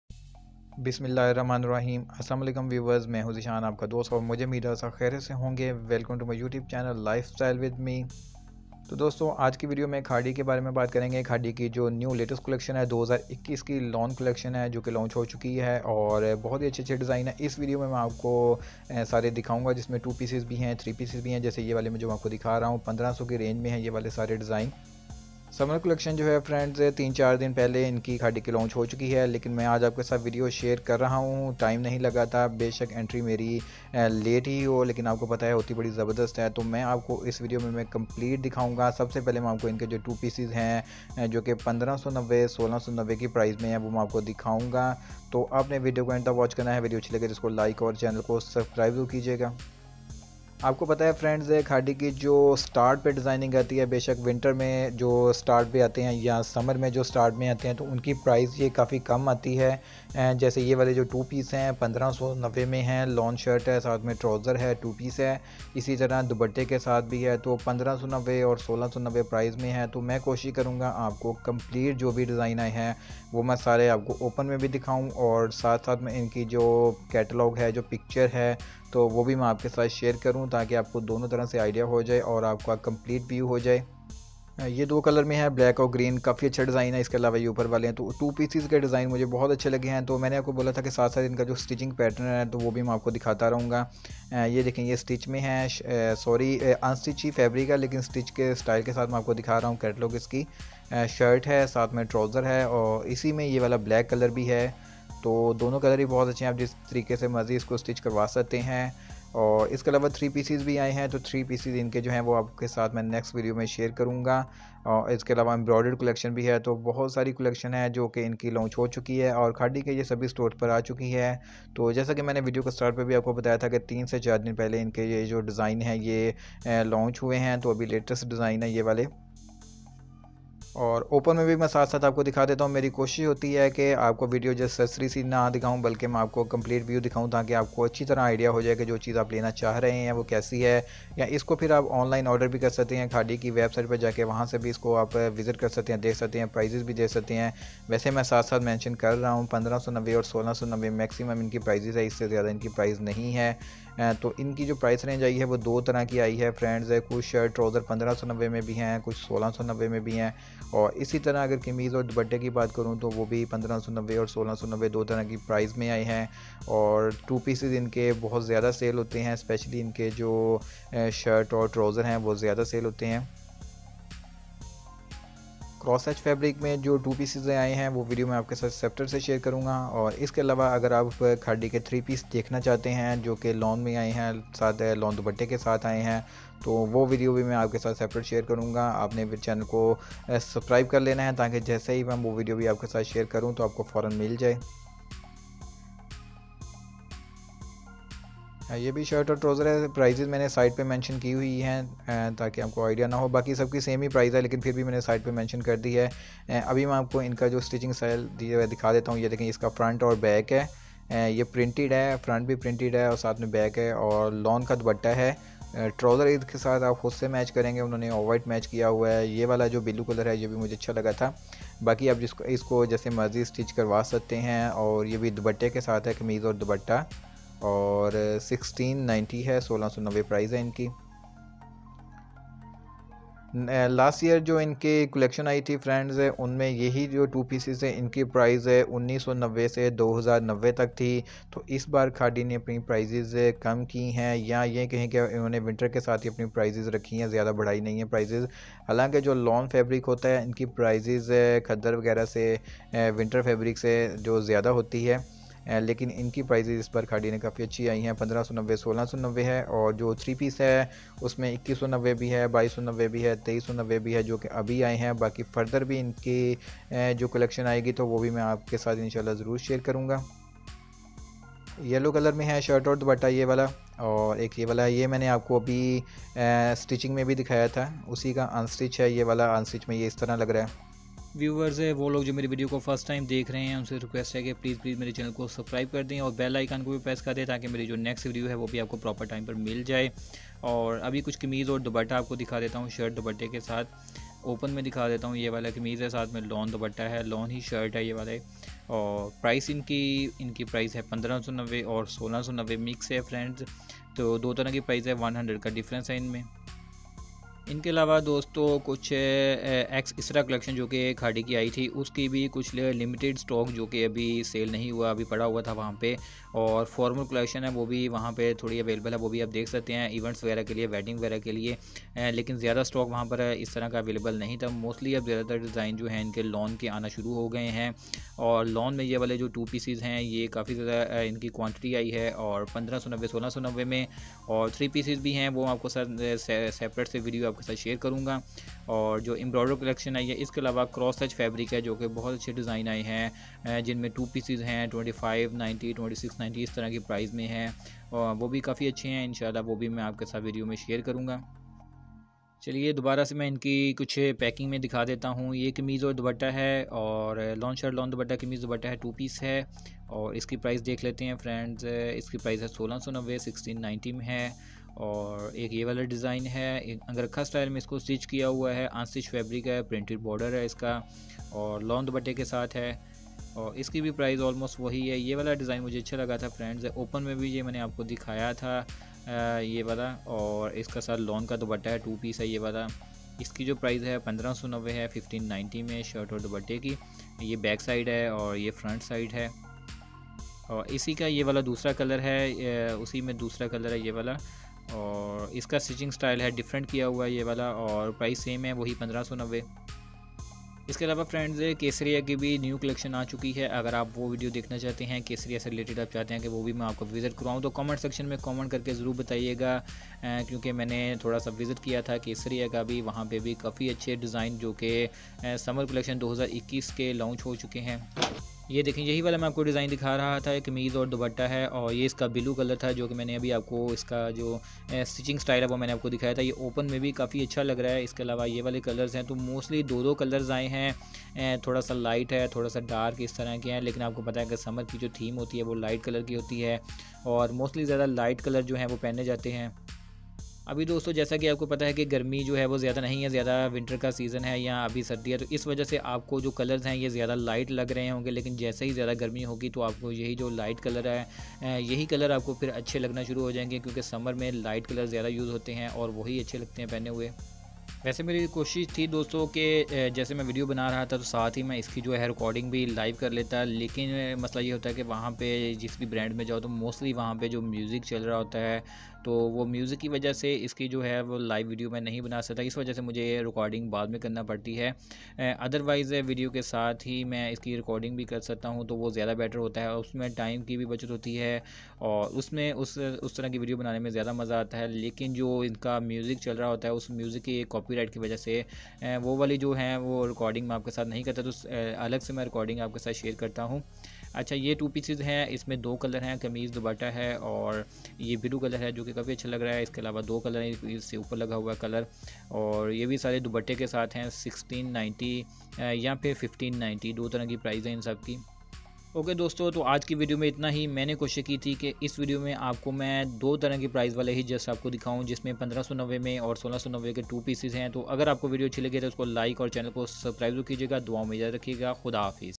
Khaadi new lawn 2021 | khaadi shop visit | zeeshan vlogs | khaadi online
In this video I will show you the latest khaadi collection in 2 Pcs starting from Rs 1500 and 1600. ALLAHA HAFIZ